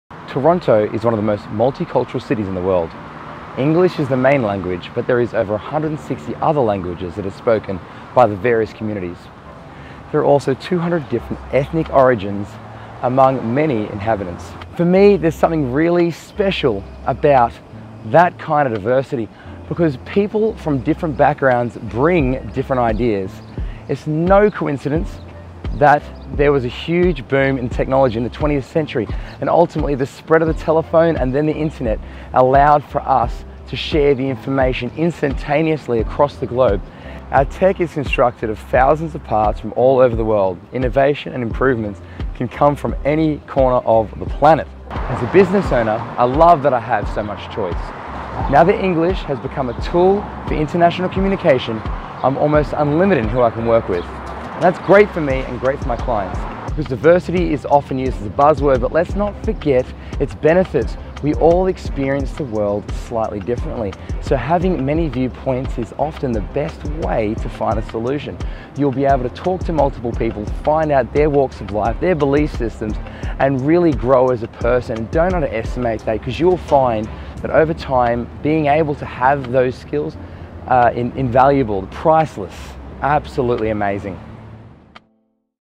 Why Is Global Communication Important to a Business?
Follow us to stay updated on what we're doing next!

Chapters
00:00 - One of the multicultural cities in the world
00:29 - Technology that allowed us to share information instantaneously
00:58 - Let's not forget about the benefits of diversity